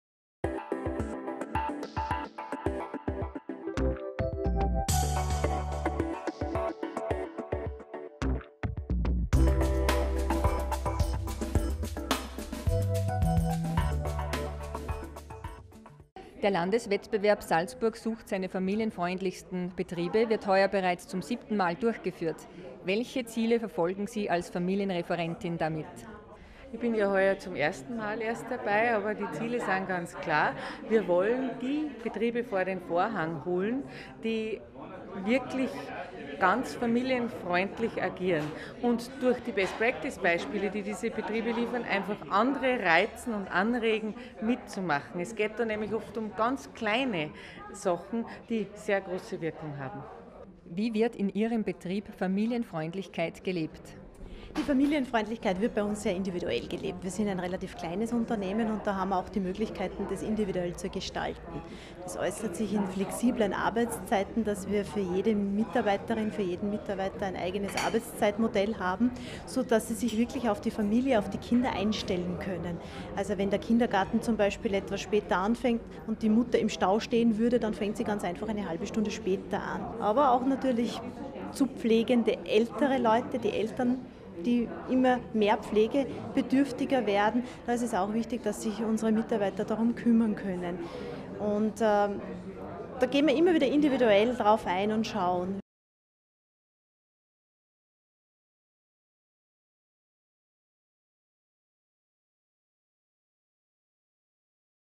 PK: Beruf und Familie vereinbaren (Land Salzburg)
Der Landeswettbewerb "Salzburg sucht seine familienfreundlichsten Unternehmen" bietet heuer schon zum siebten Mal die Möglichkeit, Betriebe, die besonders kinder- und familienfreundlich sind, vor den Vorhang zu holen. Ausgezeichnet werden jene Unternehmen, die für ihre Mitarbeiter neue und kreative Wege in der Vereinbarkeit von Familie und Beruf gefunden haben. Im November 2011 erfolgt die Prämierung durch Familienreferentin Dr. Tina Widmann und Wirtschaftsreferent Dr. Wilfried Haslauer.